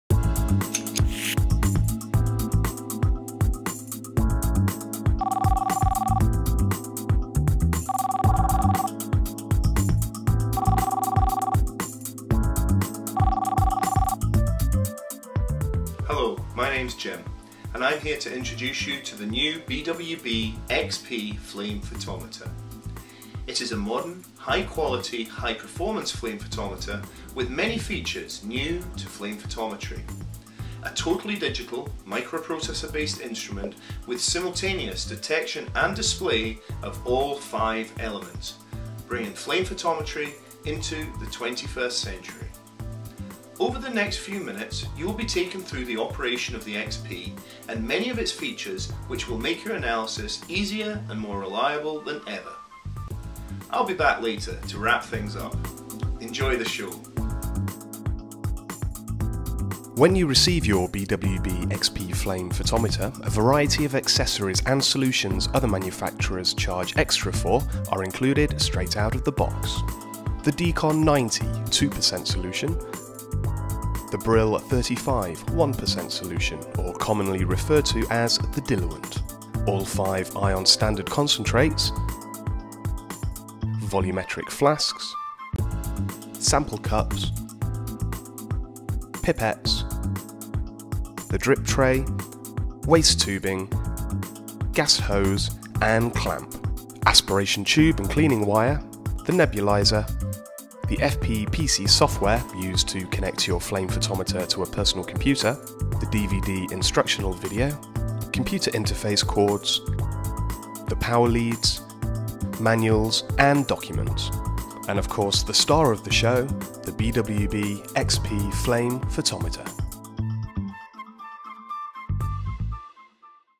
Chapter 1 - Introduction to the BWB XP Flame Photometer
In the first chapter of this new tutorial series we introduce you to your new BWB XP Flame Photometer and highlight the various accessories and solutions provided as standard with all BWB XP Flamephotometers. Chapter 2